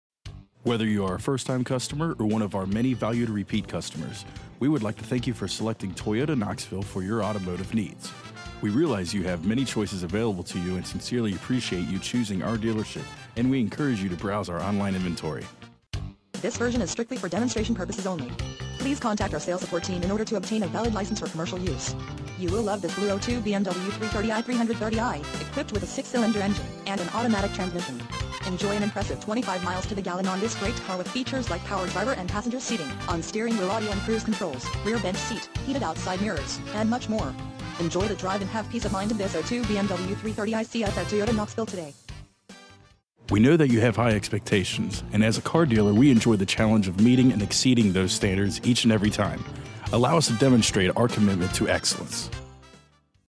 BMW I, Toyota Knoxville- Knoxville, TN 37922
BMW I, Toyota Knoxville check out this BLUE 2002 BMW 330I, equipped with a 6 Cyl. engine and an automatic transmission with 154,758 miles. enjoy an impressive 25 miles to the gallon on this great car with features like power driver and passenger seating, on steering wheel audio and cruise controls, rear bench seat, heated outside mirrors, and much more. Enjoy the drive and have peace of mind in this 2002 BMW 330I. see us at Toyota Knoxville today!

miles- 154,758

vin- WBAEV53472KM23150

Toyota Knoxville

10415 Parkside Dr